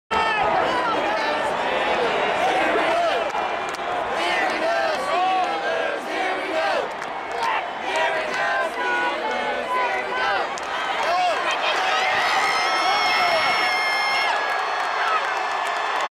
Nice cut by Mendy
Again. sorry for the shakiness. The people in front of me started to stand so I did as well